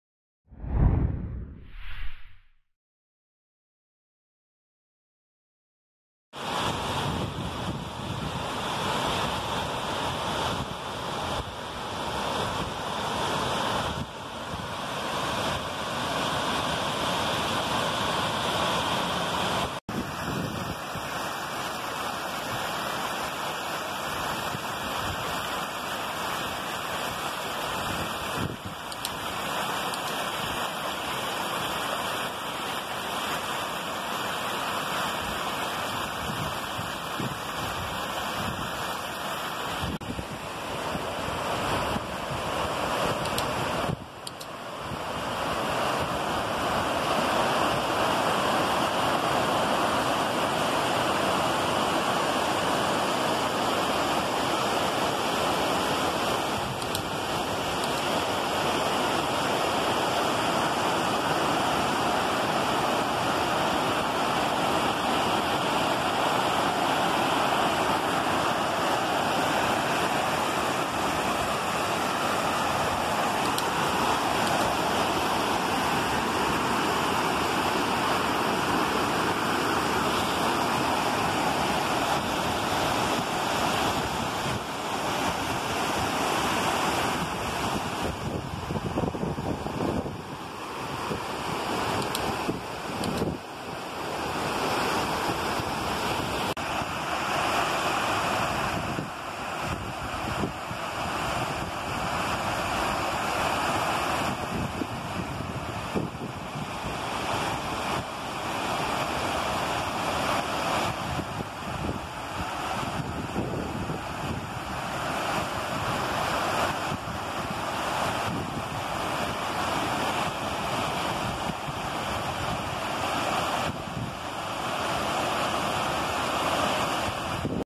Barragem do Trici Continua transbordando em Tauá - CE ( 29/04/20 )
Barragem do trici que fica localizado no interior do município de Tauá no sertão dos Inhamuns no Ceará ainda continua sangrando e a água desce para o açude Arneiroz 2

Faça uma visita ao Canal do meu amigo linda imagem

TV TRÊS RIOS WEB CRUZETA - RN